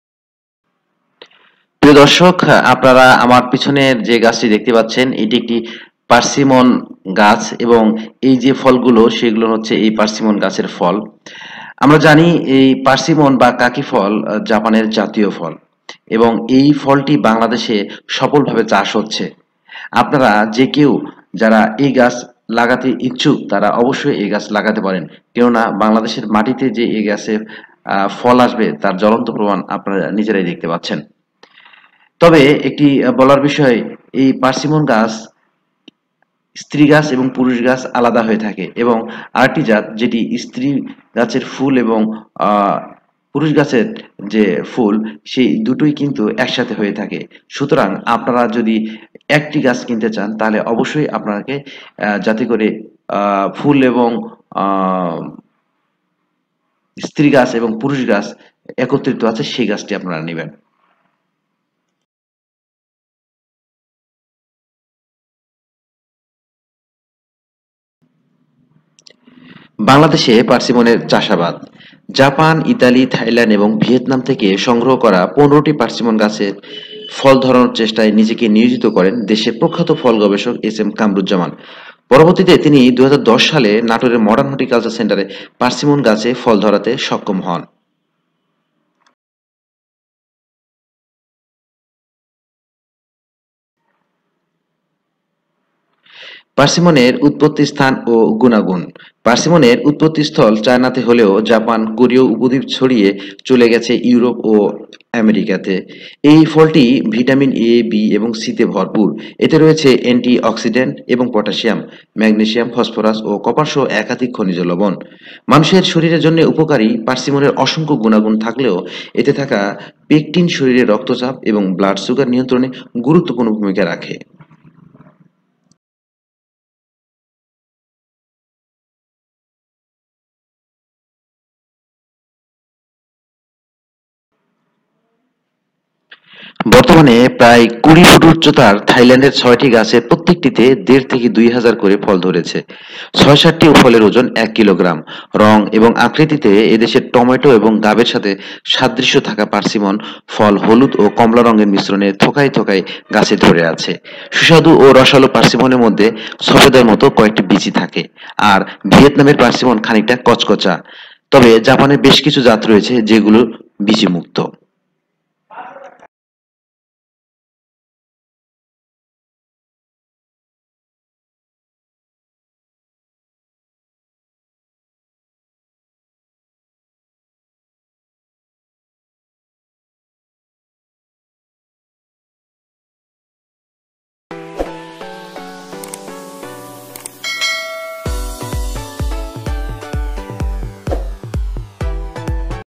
বিদেশী পার্সিমন ফল চাষ হচ্ছে বাংলাদেশে || Persimmon Fruits in Bangladesh
পার্সিমন ফল বা কাকী ফল। এই ফলটিই হচ্ছে জাপানের জাতীয় ফল। সারা পৃথিবীতেই এই ফলের চাহিদা অনেক। অত্যন্ত পুষ্টিকর এবং উচ্চ মূল্যের ফল এটি। আমাদের দেশে সাধারণত সুপার শপগুলোতে এই ফল পাওয়া যায়। যেমন অ্যাগোরা, স্বপ্ন, প্রিন্স ইত্যাদি। অন্যান্য বিদেশী ফল যেমন আপেল, কমলা, নাশপাতি, আংগুর ইত্যাদি ফল যেমন বাংলাদেশের সকল জায়গায় পাওয়া গেলেও এই persimmon ফল সব জায়গায় পাওয়া যায় না। আমাদের দেশে যেসকল জায়গায় এই ফল পাওয়া যায় তার প্রায় সবটুকুই বিদেশ থেকে আমদানি করতে হয়। এখনো বাংলাদেশে এই ফলের বানিজ্যিকভাবে চাষ শুরু হয়নি। কেউ কেউ দু একটা গাছ শখের বশে লাগিয়েছেন। সেগুলোর কিছু গাছে ফল এসেছে আবার অনেক গাছেই ফল আসেনি।
নাটোরের সনামধন্য কৃষিবিদ জনাব এস এম কামরুজ্জামান তিনি তার নিজস্ব মডার্ন হর্টিকালচার সেন্টারে প্রায় ১০ থেকে ১৫টি এই পার্সিমন ফলের গাছ সংগ্রহ করেছেন। কয়েক বছর থেকে এই গাছগুলোতে প্রচুর পরিমান ফল আসতে শুরু করেছে। তিনি ফলের চারার বিক্রি করে থাকেন। বাংলাদেশের মাটি ও আবহাওয়া এই ফল চাষের জন্য খুবই উপযোগী। যে কেউ চাইলেই এই ফল শখের বশে কিংবা বানিজ্যিকভাবে চাষ করতে পারেন। বাজারে এই ফলের মূল্য কেজি প্রতি ৫০০ টাকা থেকে ১০০০ টাকা পর্যন্ত।

 উৎপত্তি ও পুষ্টিগুন:
পার্সিমনের উৎপত্তিস্থল চায়নাতে হলেও জাপান, কোরীয় উপদ্বীপ ছাড়িয়ে চলে গেছে ইউরোপ ও আমেরিকাতে। এই ফলটি ভিটামিন এ, বি এবং সি’তে ভরপুর। এতে রয়েছে এন্টি অক্সিডেন্ট এবং পটাশিয়াম, ম্যাগনেশিয়াম, ফসফরাস ও কপারসহ একাধিক খনিজ লবণ। মানুষের শরীরের জন্যে উপকারী  অসংখ্য গুনাগুণ থাকলেও এতে থাকা পেকটিন শরীরের রক্তচাপ এবং ব্লাড সুগার নিয়ন্ত্রণে গুরুত্বপূর্ণ ভূমিকা রাখে।